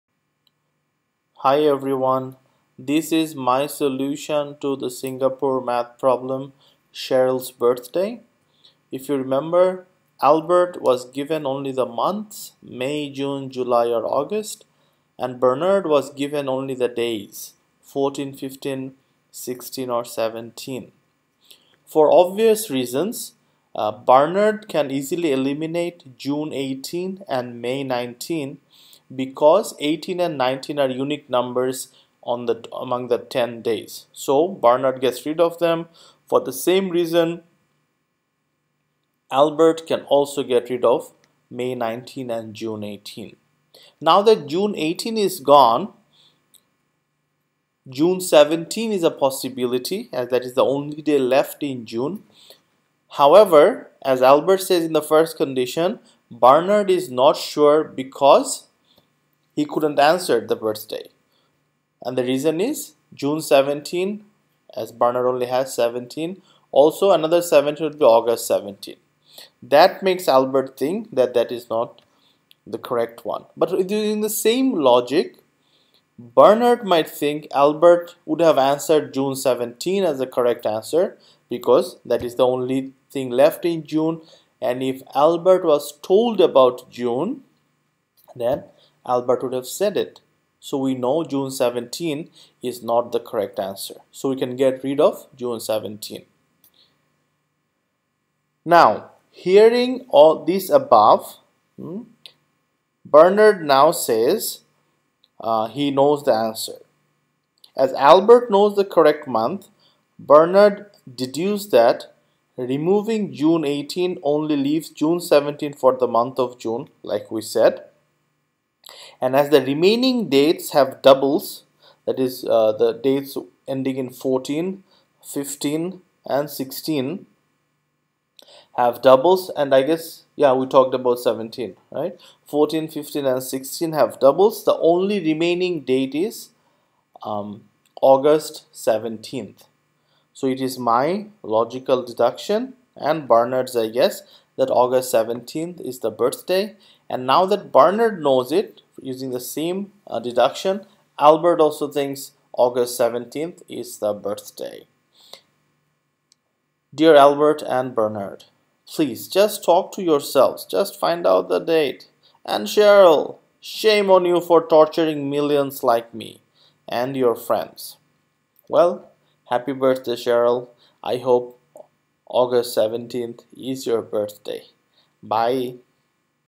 SPOILER ALERT How to solve Albert, Bernard and Cheryl's birthday maths problem
How to solve Albert, Bernard and Cheryl's birthday maths problem.

The question was posted by Singapore TV presented Kenneth Kong on his Facebook page.